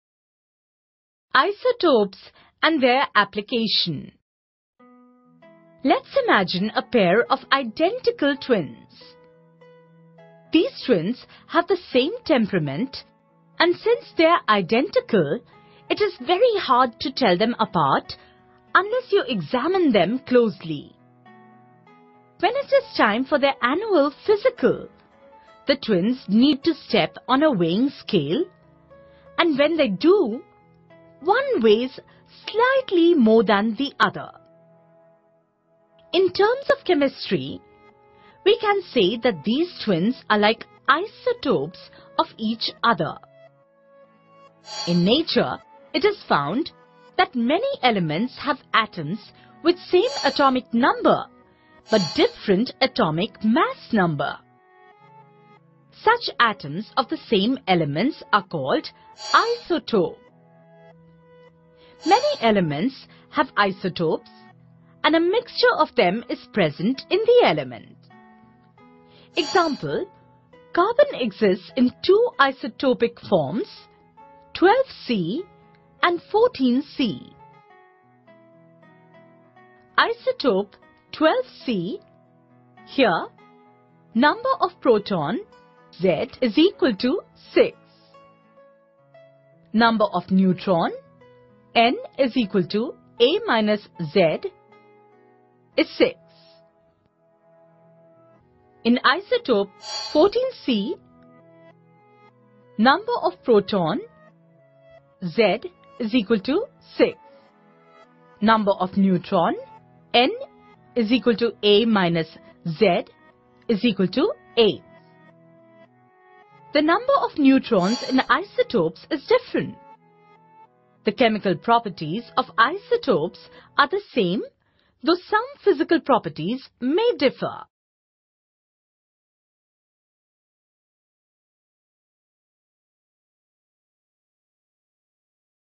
Isotopes and Their Application | Science
Let's imagine a pair of identical twins. These twins have the same temperament, and since they're identical, it is very hard to tell them apart unless you examine them closely. When it is time for their annual physical, the twins need to step on a weighing scale, and when they do, one weighs slightly more than the other. In terms of chemistry, we can say that these twins are like isotopes of each other.